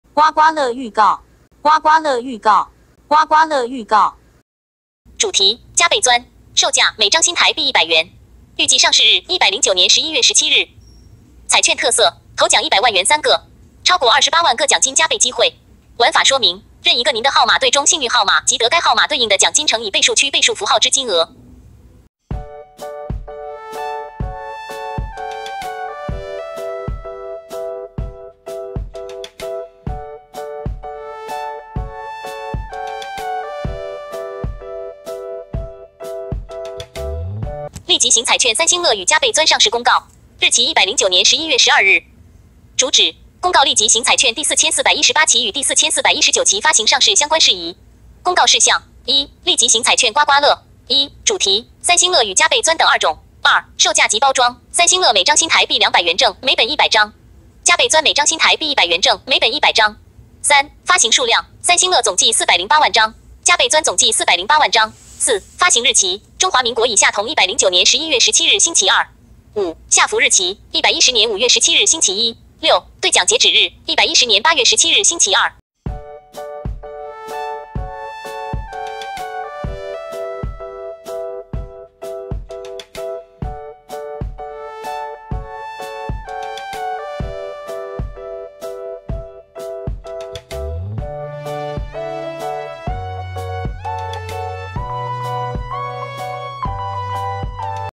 [刮刮樂] 加倍鑽刮刮樂，預告和說明，預計上市日：109年11月17日 [NT$100][頭獎100萬元3個][2020刮刮樂][加倍鑽][刮刮樂預告]
發哥開獎

主題：加倍鑽
售價：每張新臺幣100元
預計上市日：109年11月17日

彩券特色：
頭獎100萬元3個
超過28萬個獎金加倍機會

玩法說明：
任一個「您的號碼」對中「幸運號碼」，即得該號碼對應的獎金乘以「倍數區」倍數符號之金額